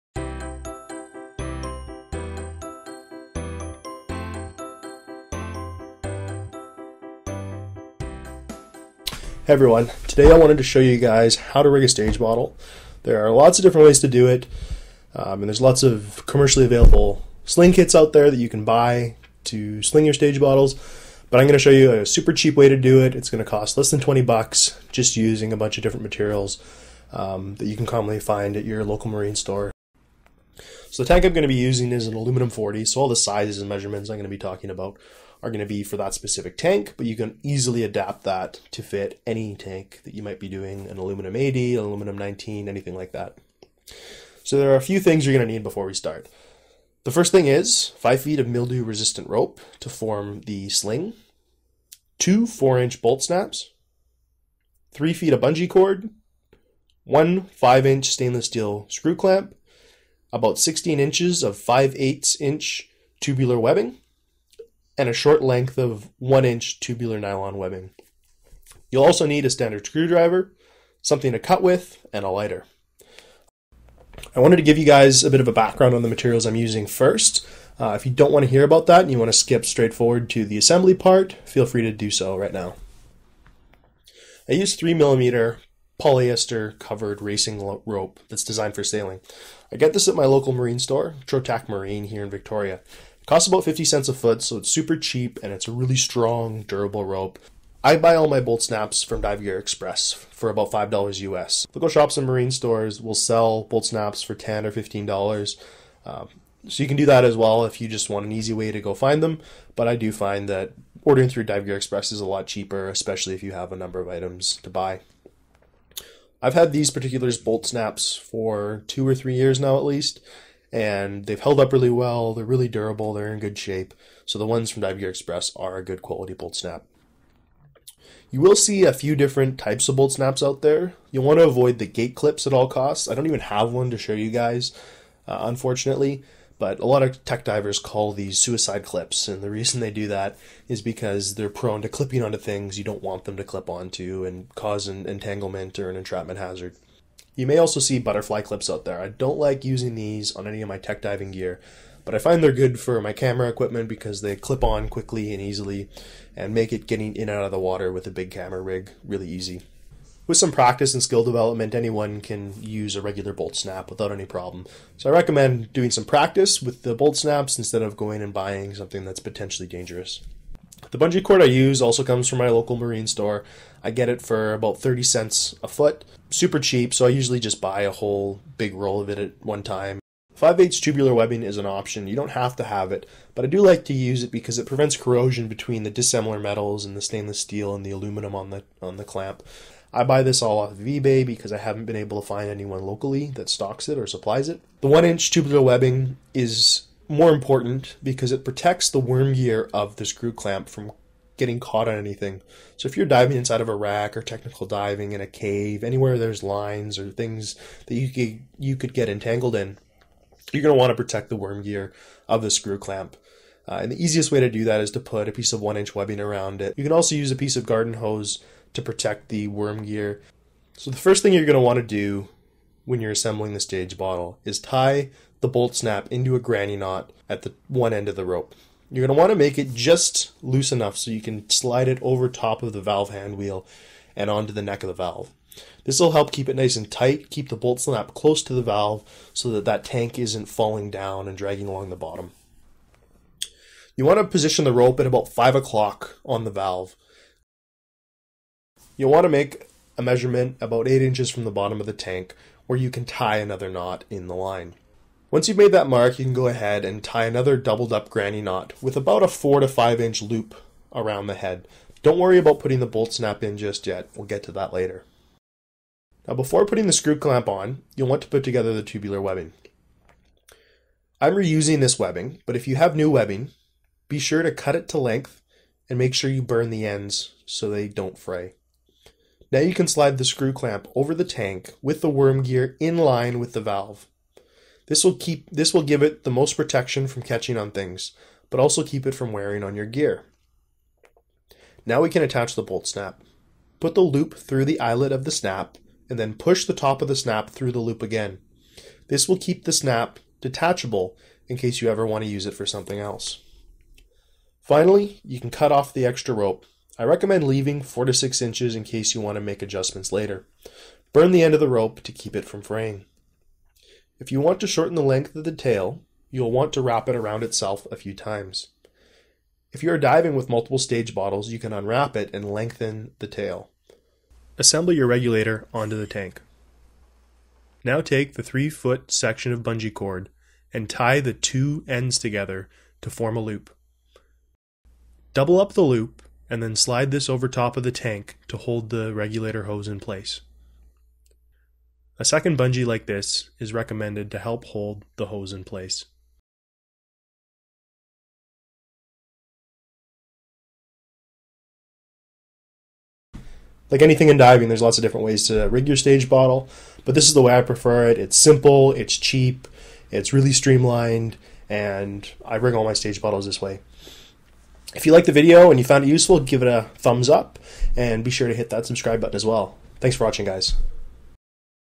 How to rig a stage bottle... for less than $20
There are plenty of commercially available sling kits out there. Cheap ones start at $60 and go up from there quickly. Instead of spending $100 on a sling kit, here is a cheap, easy and streamlined way to rig your stage bottles. Hopefully you get some useful ideas from this video.